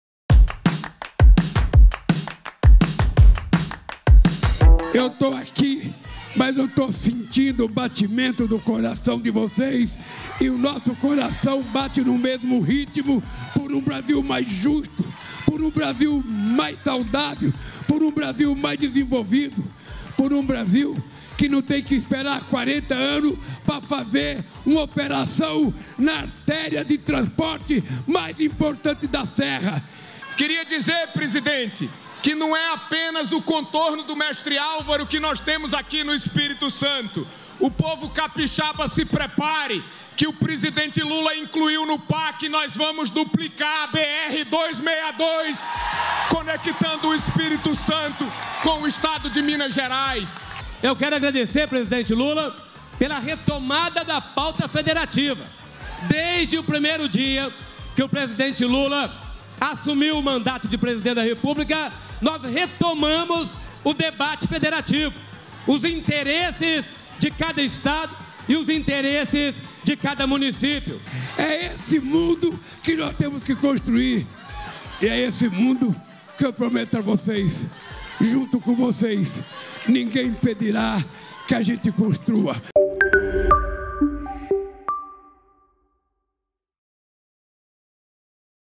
Contorno Rodoviário Mestre Álvaro
O Contorno Rodoviário Mestre Álvaro, no Espírito Santo, já está em operação, unido caminhos e sonhos, por onde passam várias riquezas do nosso país. Os investimentos do Novo PAC levam desenvolvimento para os estados e municípios, fortalecendo o pacto federativo e melhoram a vida das pessoas.

🎥 Audiovisual/PR